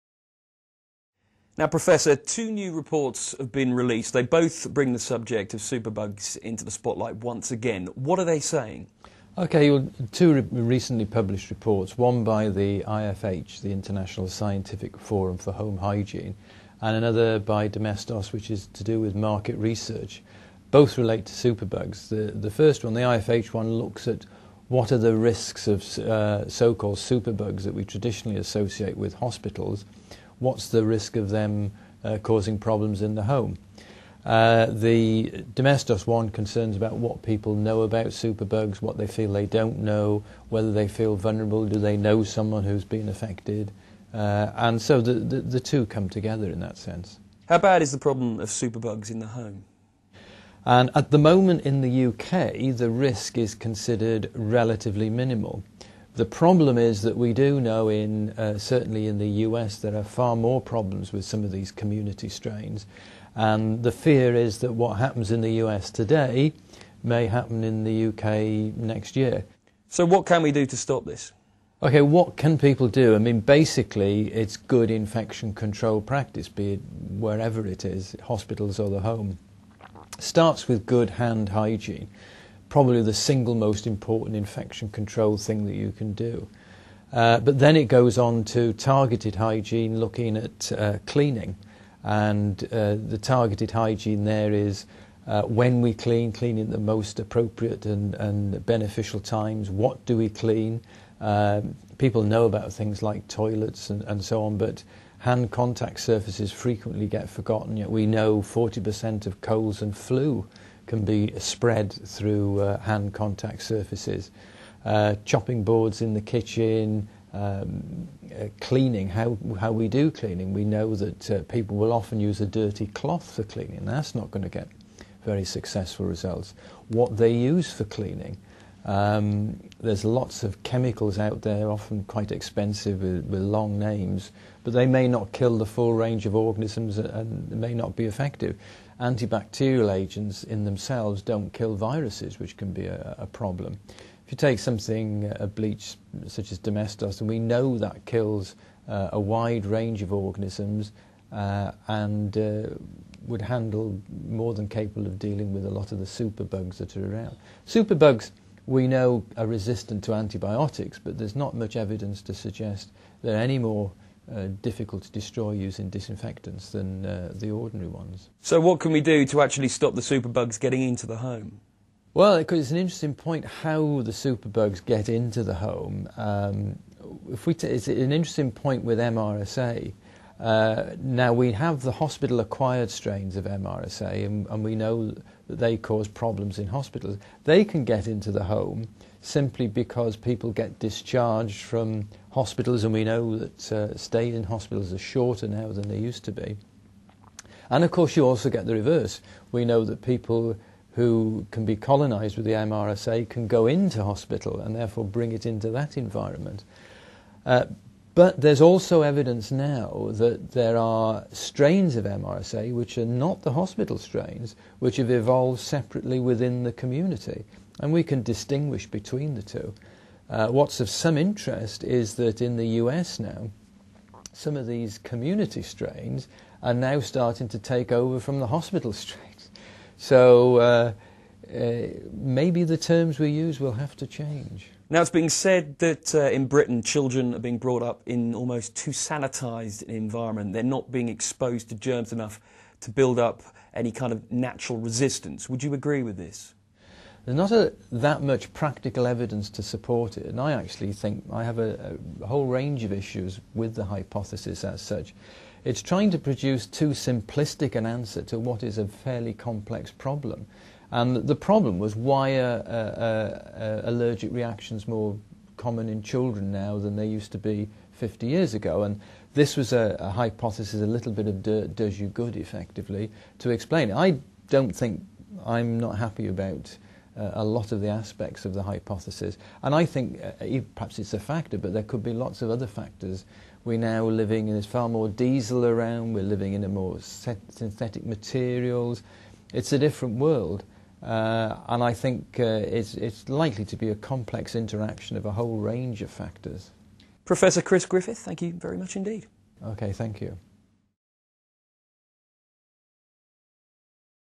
Superbugs Creeping into our Homes!!!
Superbugs: Two new reports reveal the risk of them moving out of the hospitals and into our homes.

The first- by the International Scientific Forum on Home Hygiene (IFH)- explores the risk of MRSA and the like becoming a fixture in our domestic lives.

The second, commissioned by leading infection and hygiene experts on behalf of Domestos explores how people can protect themselves from superbugs.

In this interview , Professor Chris Griffith, consumer hygienist and microbiologist at the University of Wales examines the research, talks about the risk of superbugs, how to fight them, and whether our children are being overprotected from germs.